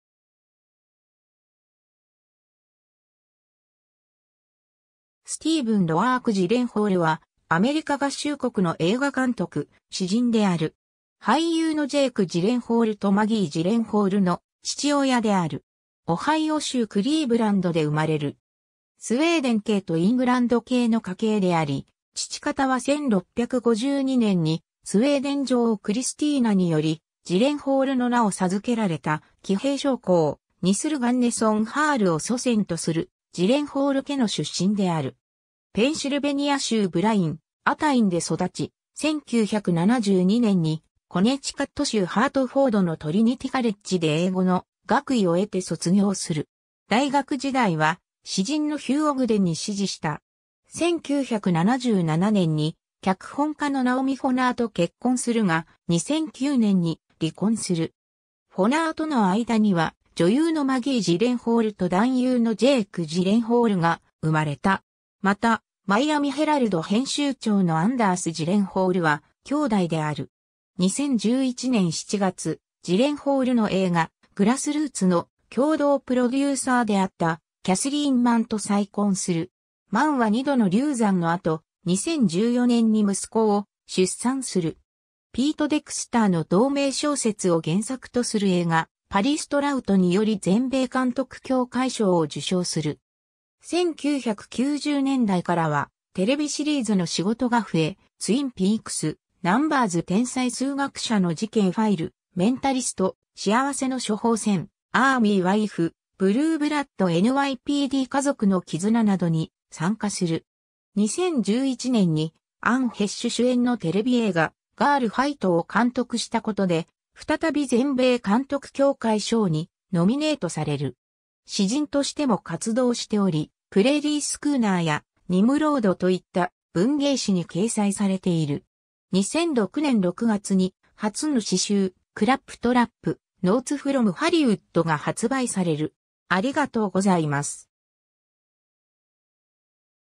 スティーヴン・ジレンホール
スティーヴン・ロアーク・ジレンホール（Stephen Roark Gyllenhaal, [ˈdʒɪlənhɔːl]; 1949年10月4日 - ）は、アメリカ合衆国の映画監督、詩人である。
俳優のジェイク・ジレンホールとマギー・ジレンホールの父親である。
 オハイオ州クリーブランドで生まれる。
スウェーデン系とイングランド系の家系であり、父方は1652年にスウェーデン女王クリスティーナにより「ジレンホール」の名を授けられた騎兵将校ニスル・ガンネソン・ハール（Nils Gunnesson Haal）を祖先とするジレンホール家の出身である。
ペンシルベニア州ブライン・アタインで育ち、1972年にコネチカット州ハートフォードのトリニティ・カレッジで英語の学位を得て卒業する。
大学時代は詩人のヒュー・オグデンに師事した。
 1977年に脚本家のナオミ・フォナーと結婚するが、2009年に離婚する。
フォナーとのあいだには女優のマギー・ジレンホールと男優のジェイク・ジレンホールが生まれた。
また『マイアミ・ヘラルド』編集長のアンダース・ジレンホールは兄弟である。
 2011年7月、ジレンホールの映画『Grassroots』（2012年）の共同プロデューサーであったキャスリーン・マンと再婚する。
マンは2度の流産の後、2014年に息子を出産する。
 ピート・デクスターの同名小説を原作とする映画『パリス・トラウト』（1990年）により全米監督協会賞を受賞する。
1990年代からはテレビシリーズの仕事が増え、『ツイン・ピークス』、『NUMBERS 天才数学者の事件ファイル』、『メンタリスト』、『しあわせの処方箋』、『アーミー・ワイフ』、『ブルーブラッド 〜NYPD家族の絆〜』などに参加する。
2011年にアン・ヘッシュ主演のテレビ映画『Girl Fight』を監督したことで再び全米監督協会賞にノミネートされる。
 詩人としても活動しており、『Prairie Schooner』や『Nimrod』といった文芸誌に掲載されている。
2006年6月に初の詩集『Claptrap: Notes from Hollywood』が発売される。